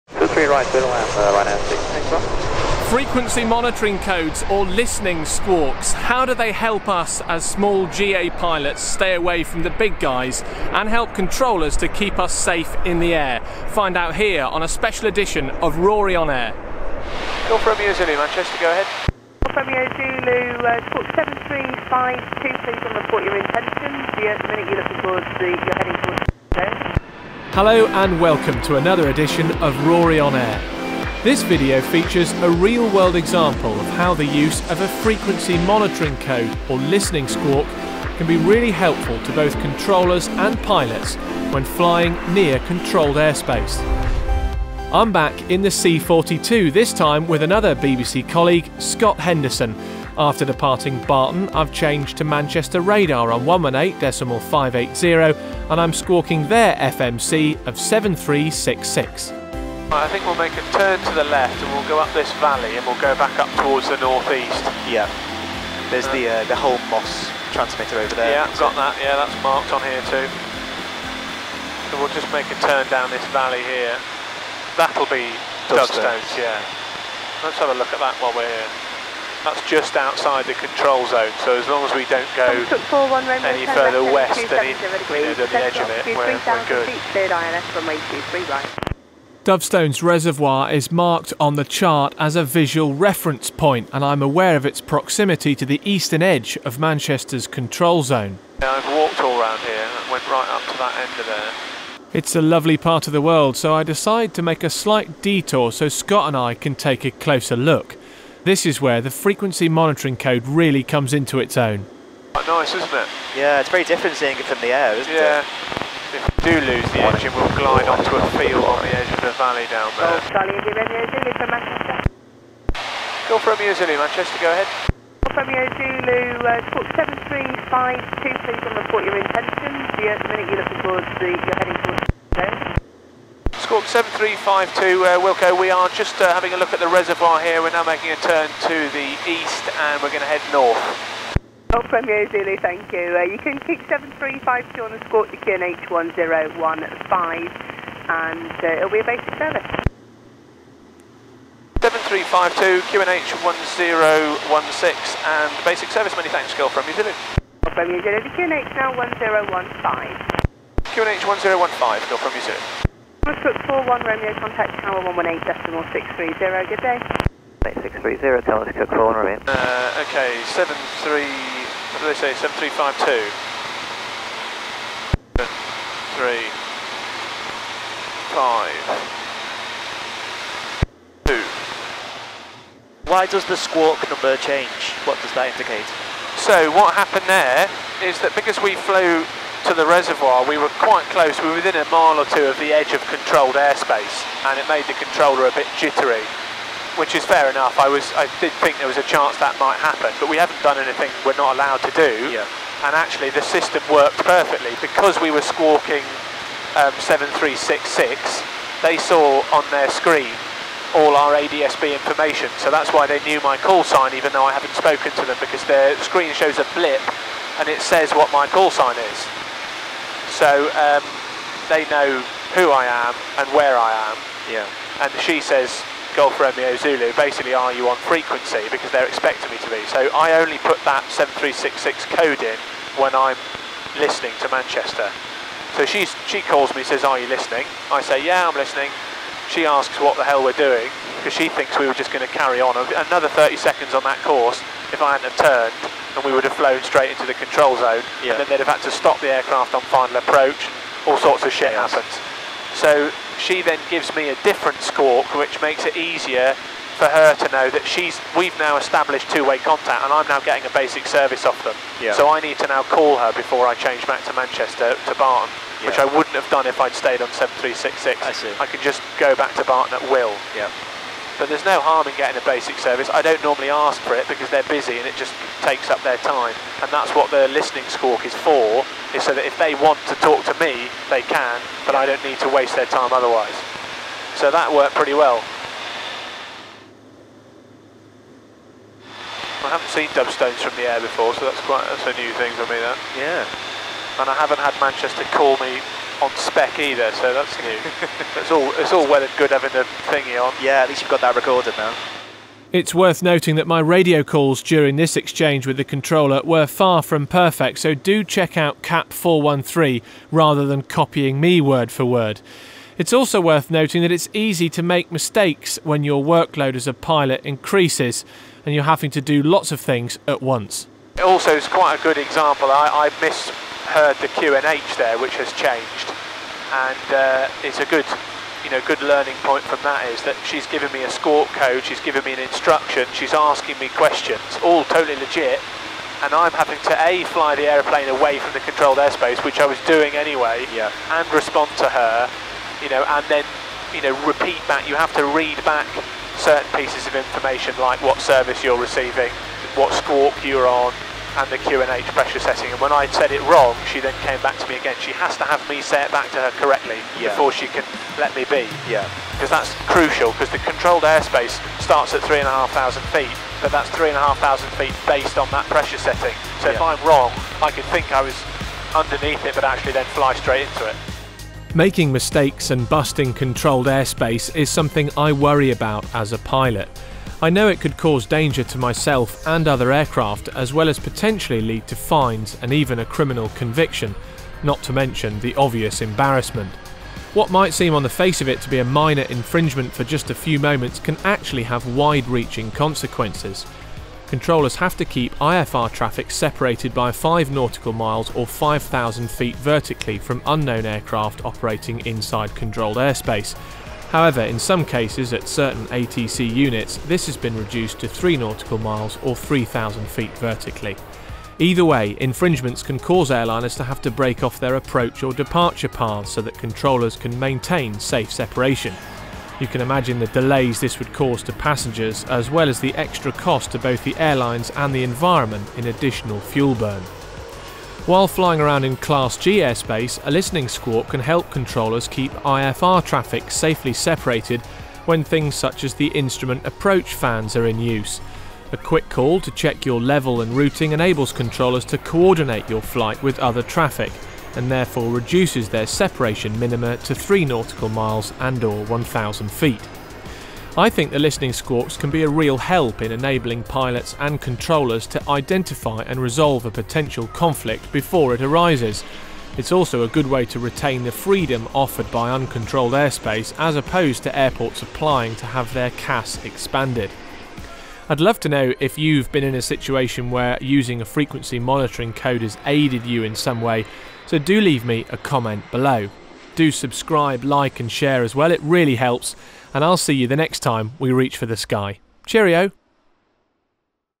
What is a 'Listening Squawk'? | Real World Example
Have you ever wondered what pilots and air traffic controllers mean when they talk about 'squawking'? You'll have heard me repeat back squawk codes like 7365 and 7000 while flying around the UK. They are 4 digit codes we put into the transponder to help give ATC a clue as to who we are and what we are doing. This video aims to explain what they are and why it's a good idea to use them. Join Scott and I as we check out Dovestones reservoir in the C42. This video features a real world example of how the Frequency Monitoring Code, or 'listening squawk' system can help stop pilots infringing controlled airspace. It also features some footage of big jets landing at Manchester airport.

Filmed with a Hero 7 Black & 2x GoPro Hero 5 Black cameras
Audio from a Zoom H1
My 'new toy' is a Feiyutech G6 Plus gimbal.